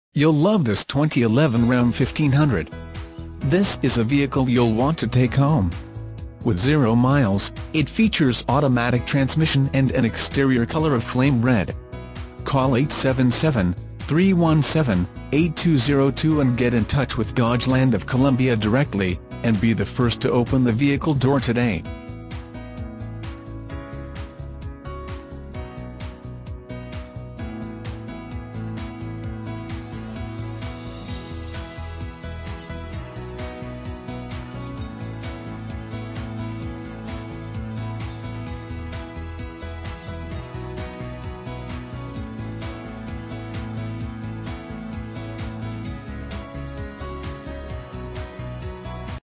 2011 Ram 1500 - Crew Cab Pickup Columbia SC 129376
2011 Ram 1500 SLT - $36990

Dodgeland of Columbia
190 Greystone Blvd

You'll love this 2011 Ram 1500. This is a vehicle you'll want to take home. With 0 miles, it features Automatic transmission and an exterior color of FLAME RED. Call and get in touch with Dodgeland of Columbia directly, and be the first to open the vehicle door today!